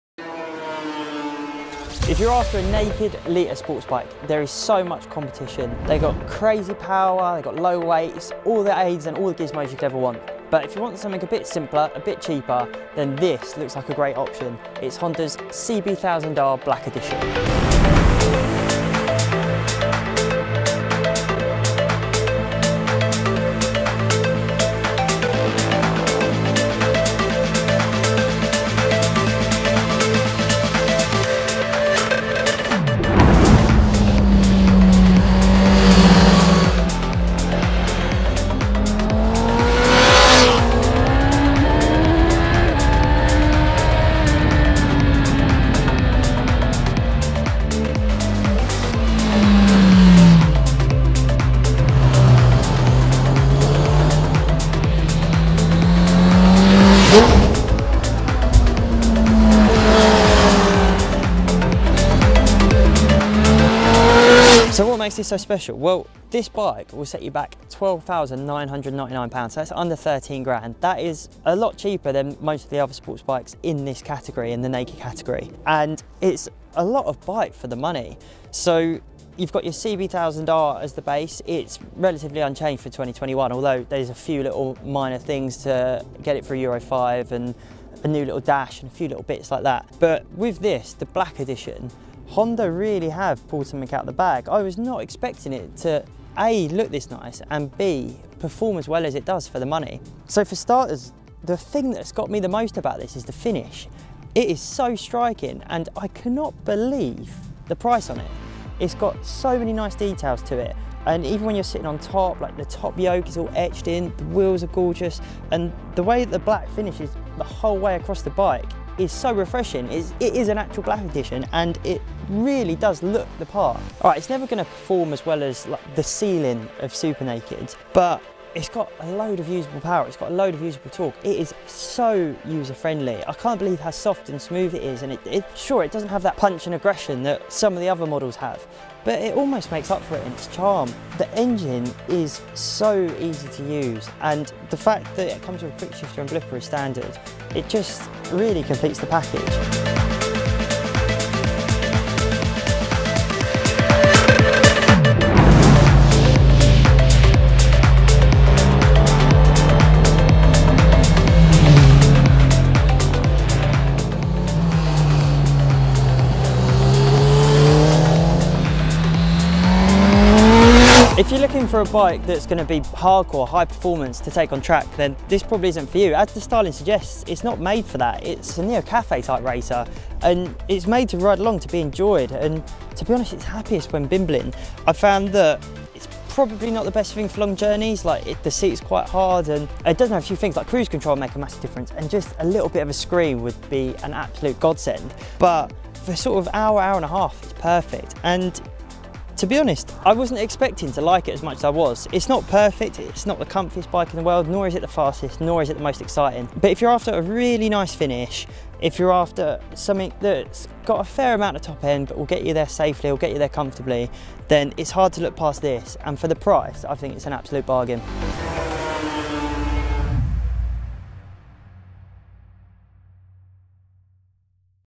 Honda CB1000R Black Edition - full road test and review
Honda's CB1000R is the 'sensible' option in the naked litre bike market.

The latest 'Black Edition' follows Honda's Neo Sports Cafe design philosophy and combines some retro/modern touches with a near 145bhp inline four engine.

It's a super naked designed for the real world, so how does it ride on UK roads? Carl Stevens took one for a spin to find out...